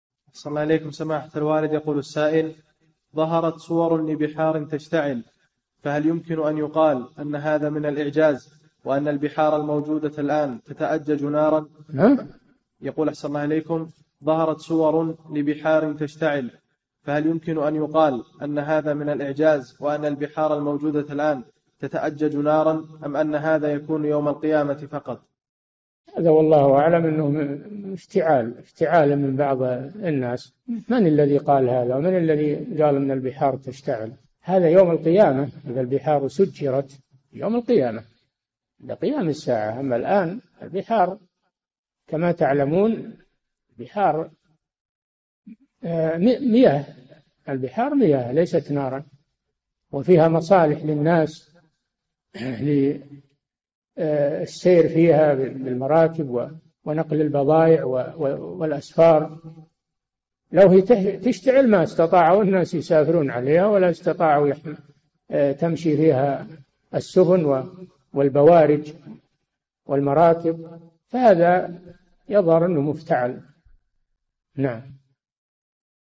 [1013- 4717] ما رأيكم فيما ظهر من صور للبحار وهي تشتعل نارا مصداقا للآية (وإذا البحار سجرت)؟
"[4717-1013] ظهرت صور لبحار تشتعل فهل يمكن أن يقال أن هذا من الإعجاز وأن البحار الموجودة الآن تتأجج نارا، أم أن هذا يكون يوم القيامة فقط؟
الجَوَابُ: هذا والله أعلم أنه من افتعال من بعض الناس من الذي قال هذا؟ ومن الذي قال إن البحار تشتعل؟ هذا يوم القيامة ﴿وَإِذَا الْبِحَارُ سُجِّرَتْ﴾ [التكوير: 6] يوم القيامة، عند قيام الساعة أما الآن البحار كما تعلمون البحار مياه ليست نارا وفيها مصالح للناس في السير فيها بالمراكب ونقل البضائع والأسفار لو هي تشتعل ما استطاع الناس يسافرون عليها ولا استطاعوا تمشي فيها السفن والبوارج والمراكب فهذا يظهر أنه مفتعل.

للاشتراك في قناة الشيخ صالح الفوزان
فتاوى المجالس في تفسير المفصل

للاستفسار والإقتراحات والدعم
رئيس الفريق
لمشروع كبار العلماء